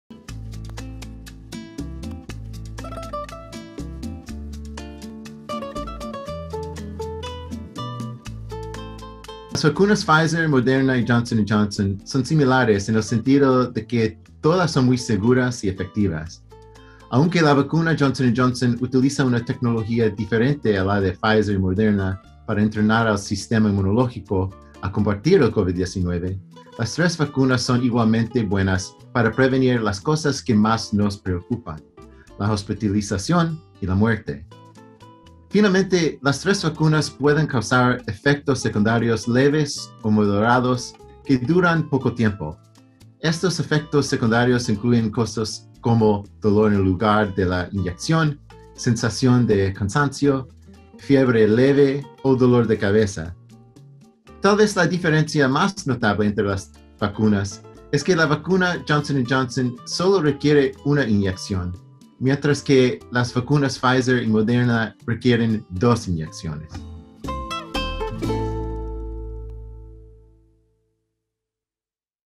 ¿En qué se parecen las vacunas y en qué se diferencian?
El Dr. Herce analiza las similitudes y diferencias en las vacunas contra el COVID-19.